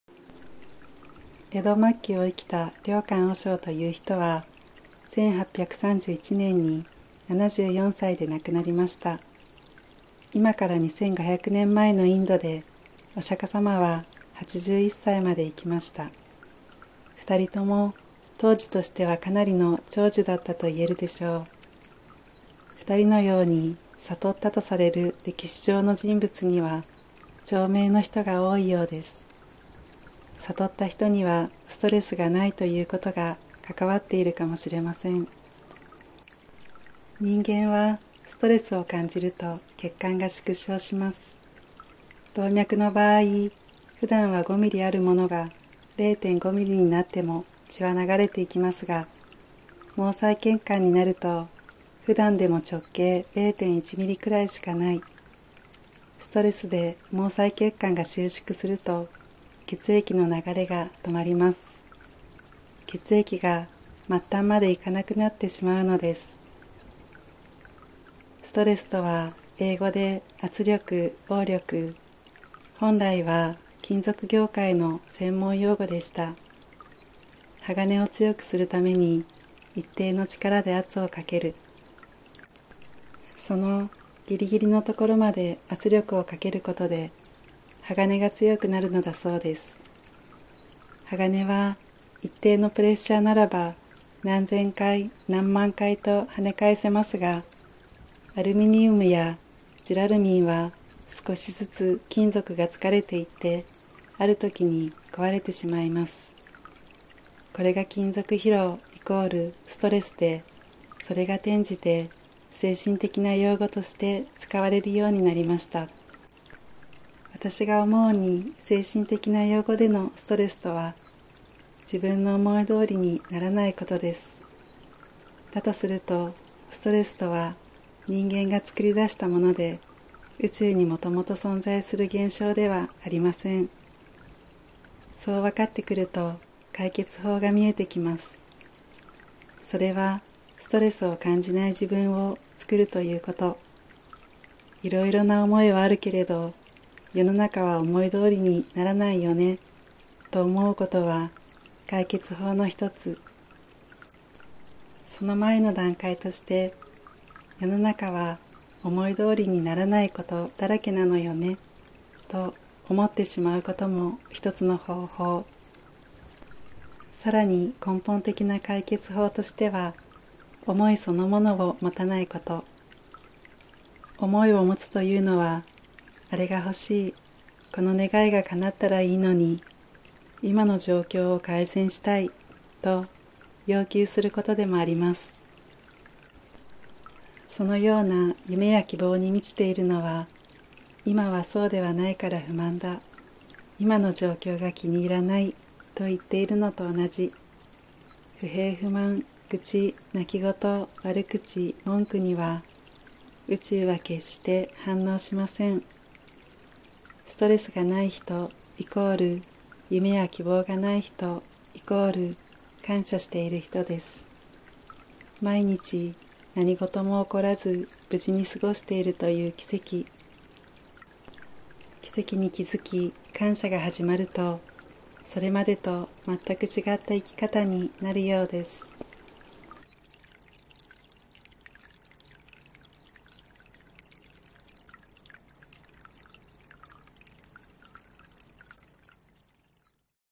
小林正観さん【ストレスとは宇宙にもともと存在する現像ではありません】
私が思うに、精神的な用語でのストレスとは「自分の思いどおりにならないこと」です。

だとすると、ストレスとは、人間が作り出したもので、宇宙にもともと存在する現象ではありません。

そうわかってくると、解決法が見えてきます。

【神様を味方にする法則　P161】より